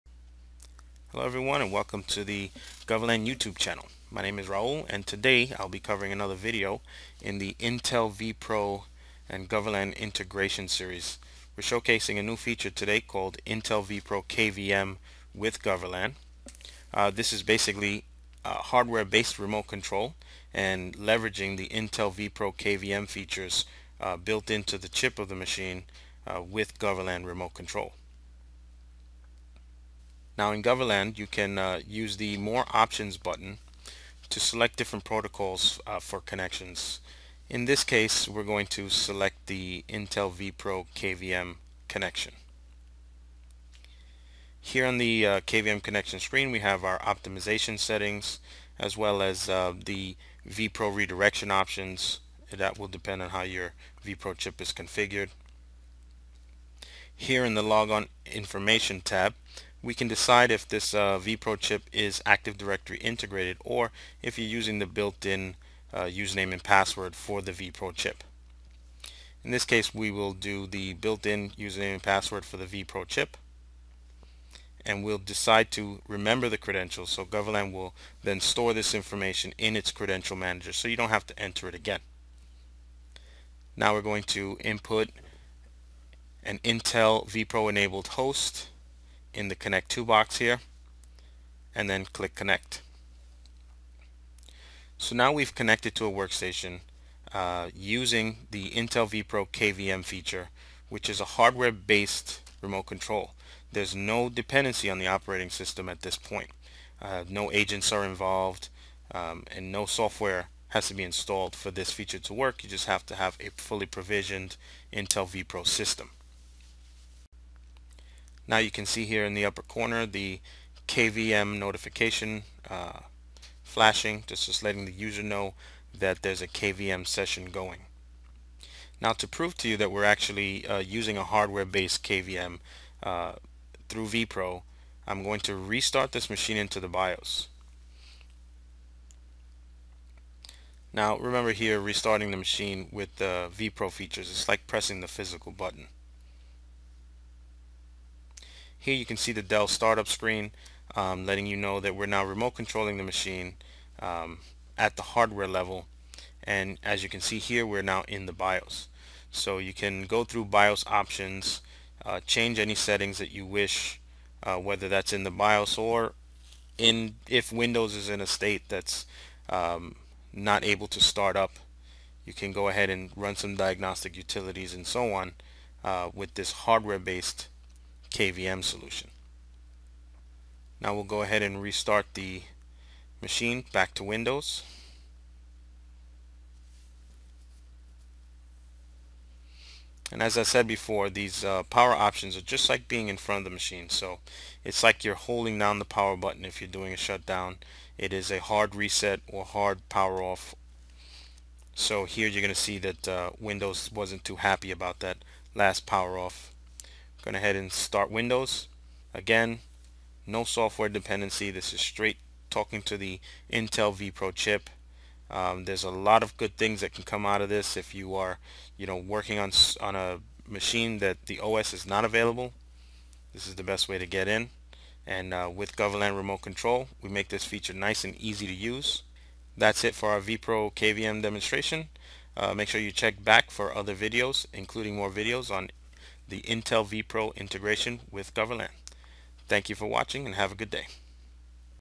Goverlan - Intel vPro Integration - Working with KVM sessions
In this video, we explore working with Out of Band KVM sessions through Goverlan Remote Control.